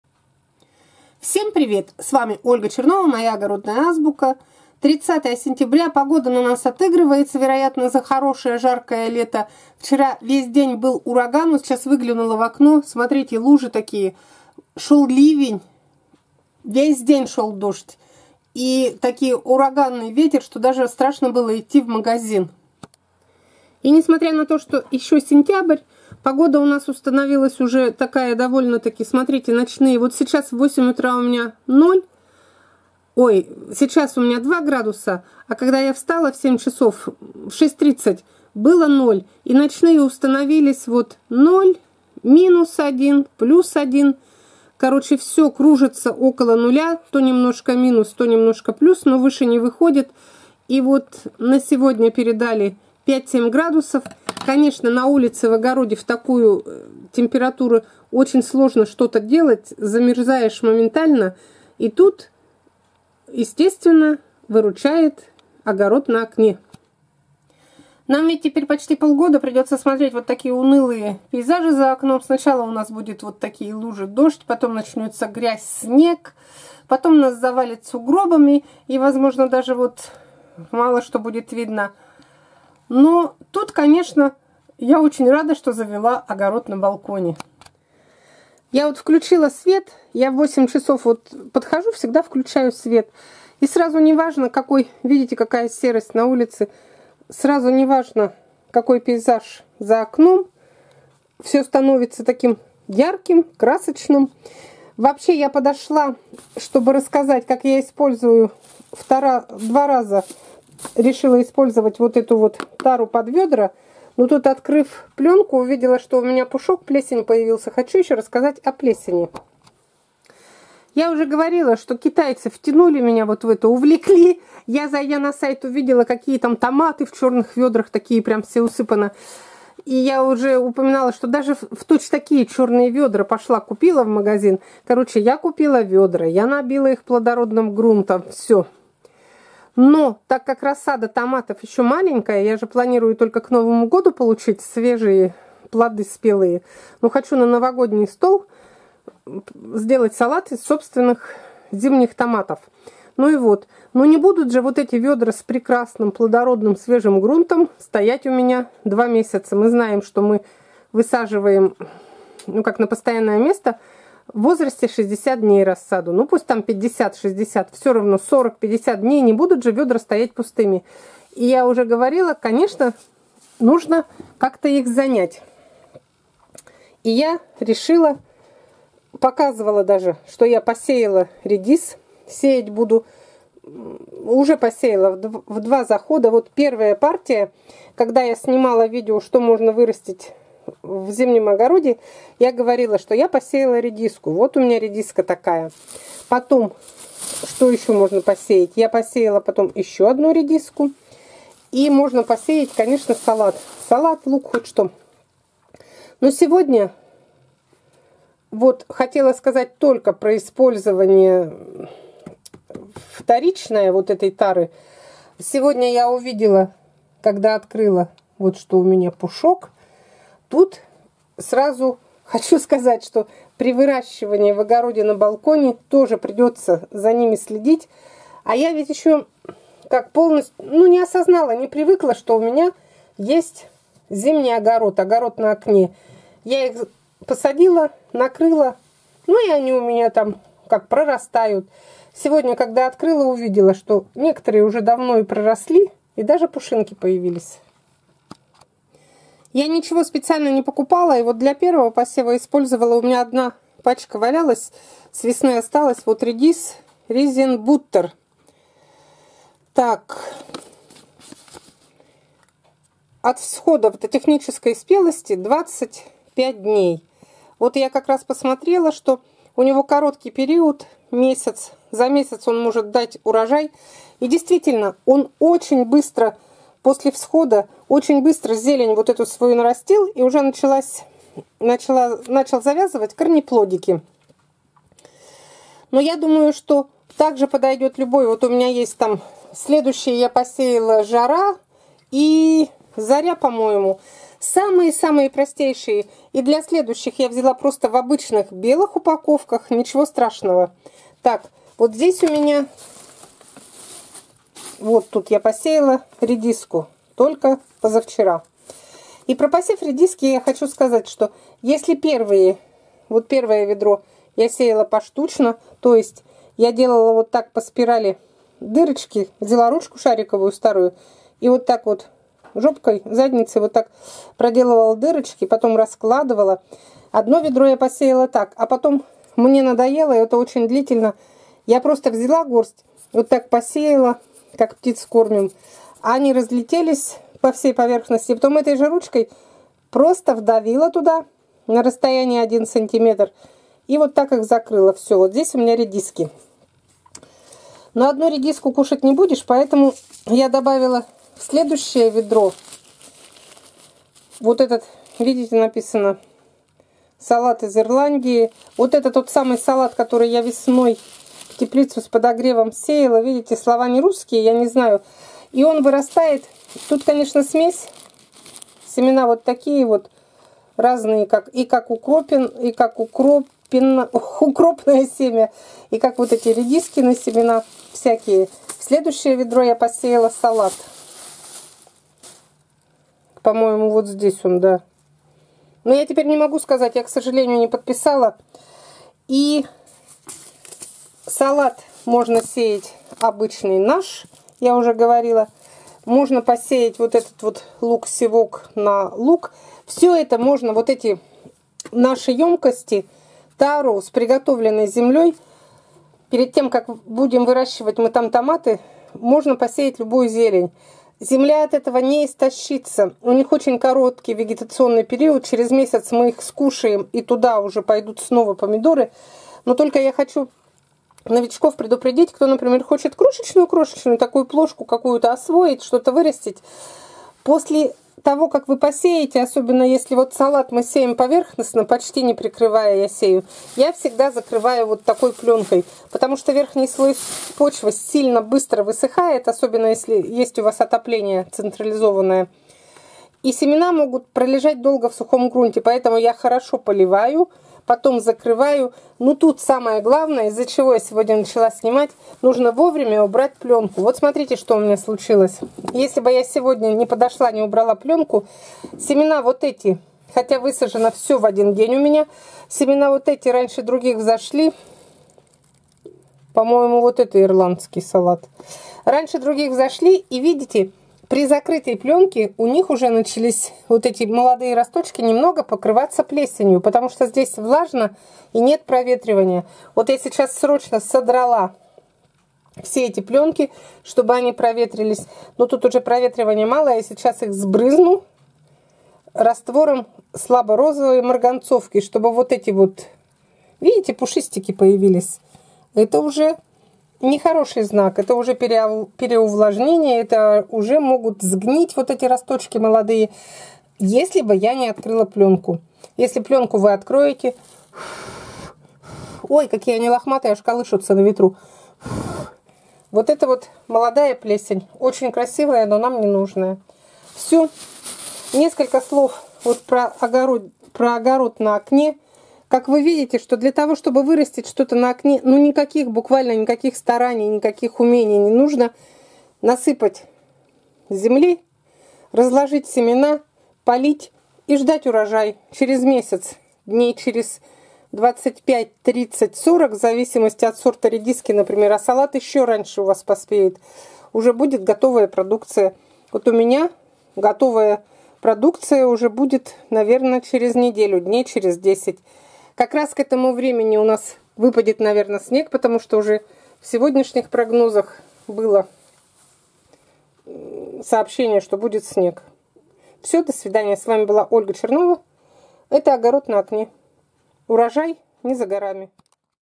ЭТОТ ГРУНТ БУДУ ИСПОЛЬЗОВАТЬ ДВАЖДЫ. Ольга Чернова.
Огород на окне не только развлечет нас в долгие зимние вечера, но и даст реальный урожай. Мой первый урожай с оконных грядок уже не за горами!